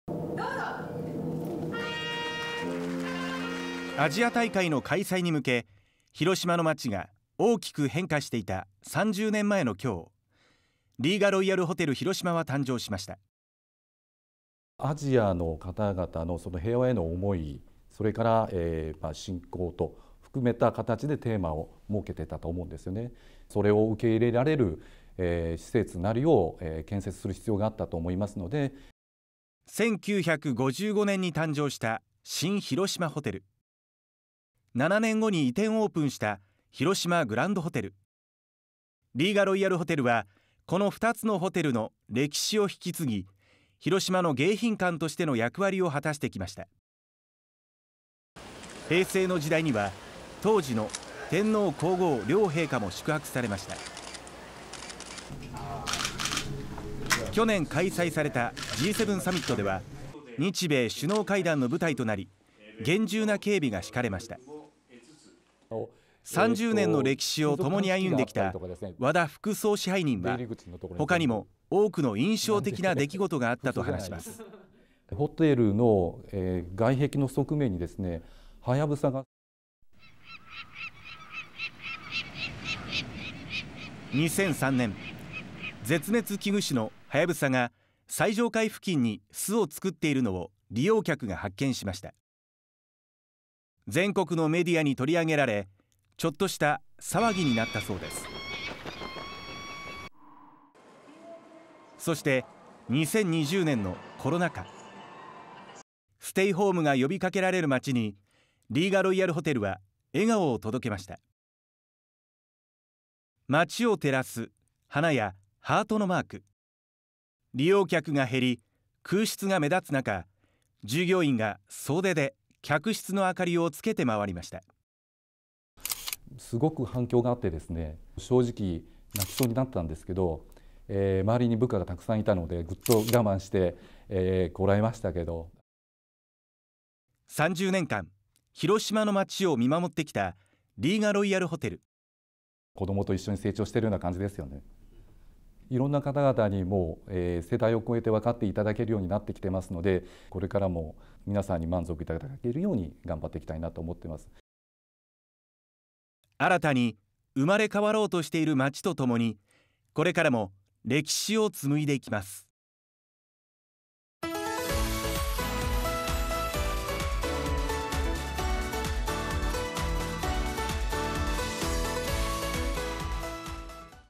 【秘話】副総支配人が語る”ハヤブサ騒ぎ” 平成には当時の天皇皇后両陛下も宿泊｜リーガロイヤルホテル広島 30年の歴史
アジア大会の開催に向け広島の街が大きく変化していた30年前の4月25日、リーガロイヤルホテル広島は誕生しました。平成の時代には、当時の天皇皇后両陛下も宿泊。去年開催されたＧ７サミットでは、日米首脳会談の舞台となり厳重な警備が敷かれました。

2024年4月25日放送